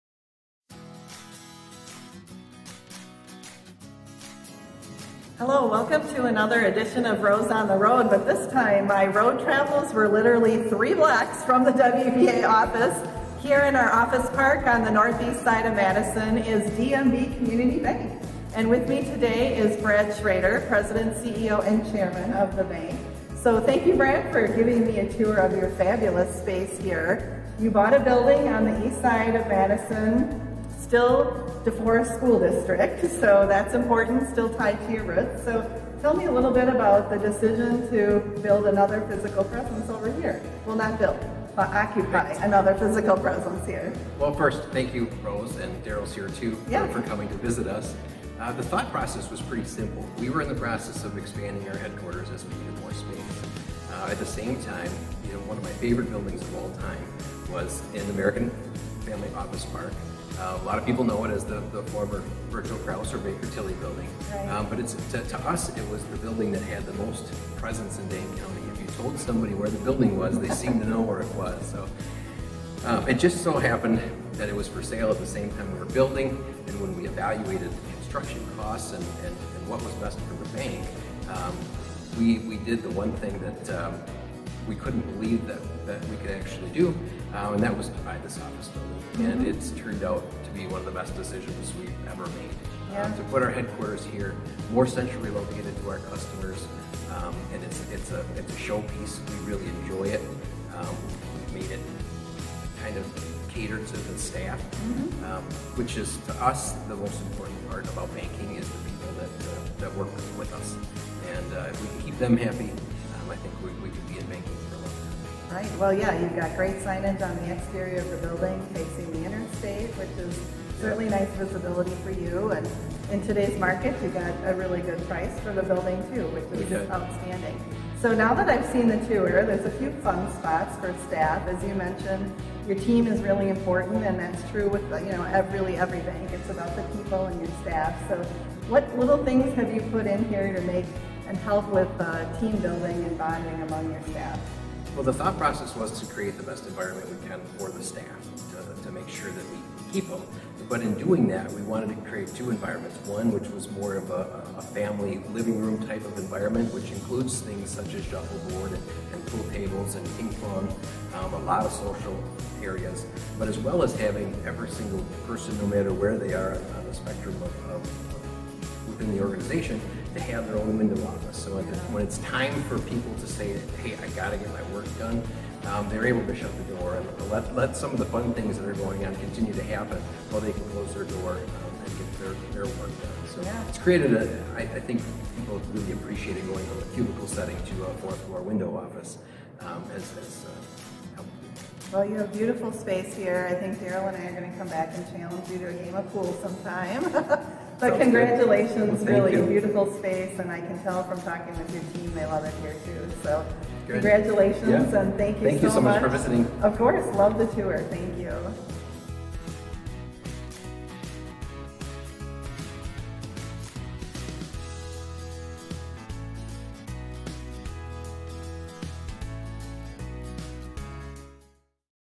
Rose on the Road: DMB Community Bank
Rose Oswald Poels, president and CEO of the Wisconsin Bankers Association (WBA), traveled just a few blocks from the WBA office last month to visit the new DMB Community Bank building. Bank President and CEO Brad Schroeder welcomed Rose to the new headquarters and talked about the additions to the space that make it a fun environment to work in for bank staff.